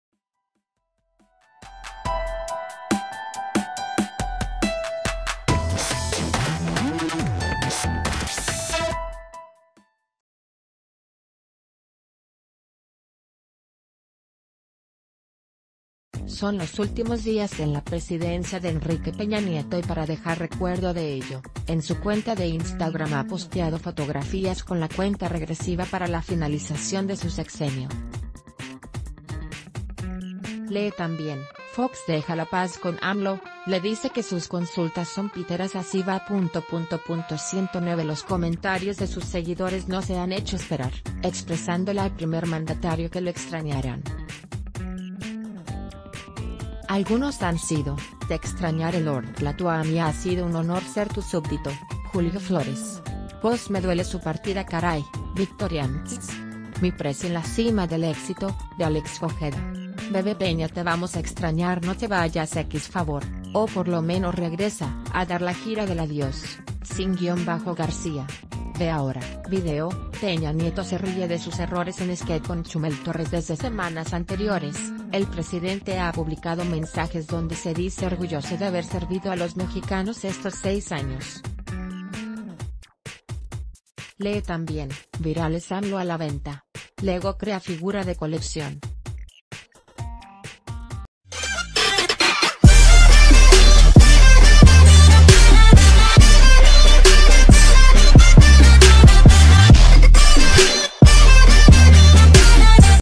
Así se despide Peña Nieto desde Instagram
Son los últimos días en la Presidencia de Enrique Peña Nieto y para dejar recuerdo de ello, en su cuenta de Instagram ha posteado fotografías con la cuenta regresiva para la finalización de su sexenio. Lee también: Fox deja la paz con AMLO, le dice que sus consultas son "piteras"Así va…109Los coment...